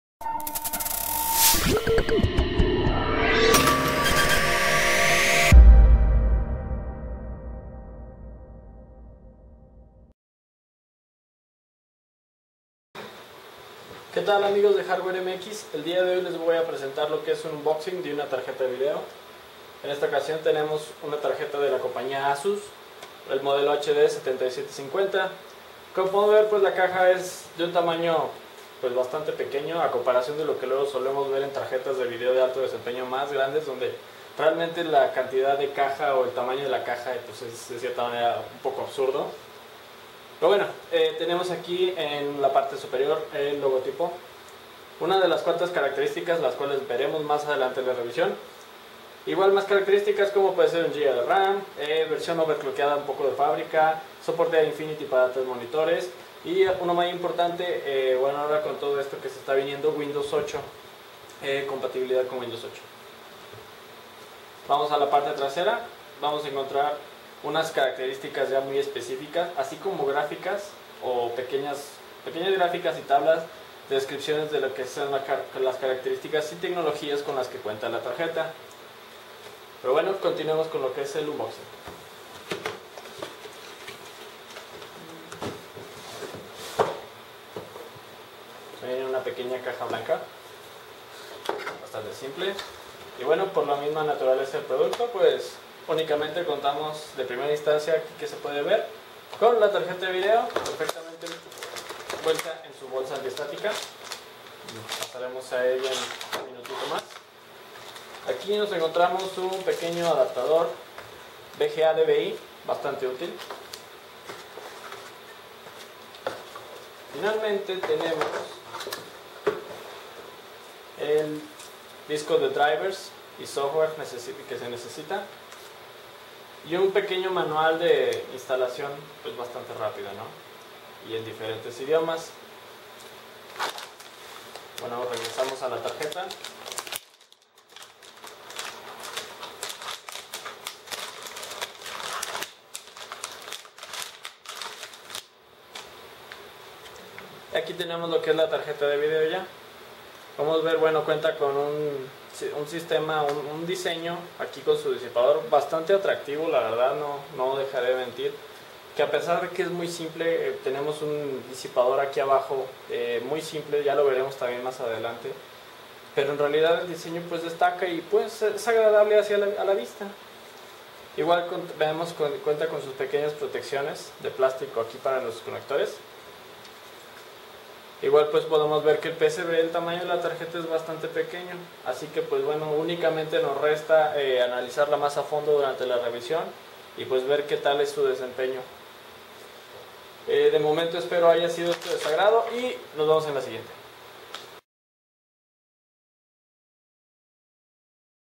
Unboxing ASUS Radeon HD 7750
Vídeo del unboxing de la tarjeta de vídeo ASUS Radeon HD 7750